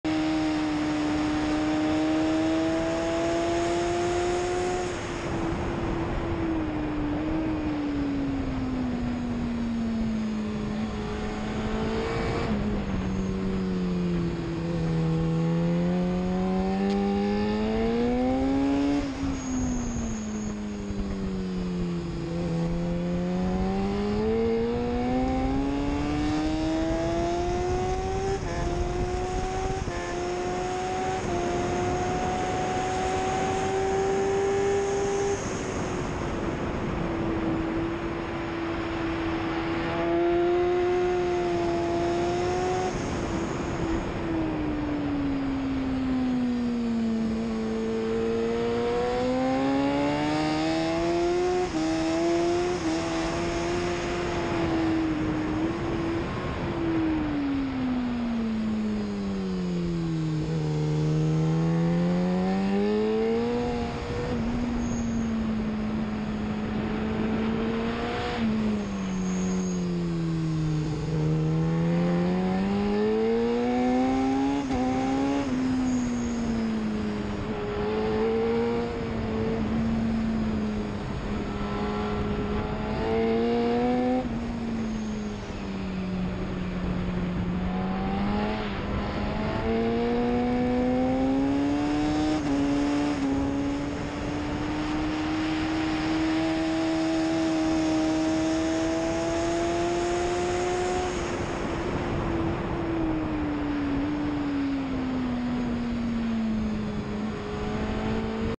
Un tour sur le grand circuit à Mirecourt en s1000rr 1.368
petit tour sur le grand circuit de mirecourt avec une s1000rr de 2011.

temps 1.368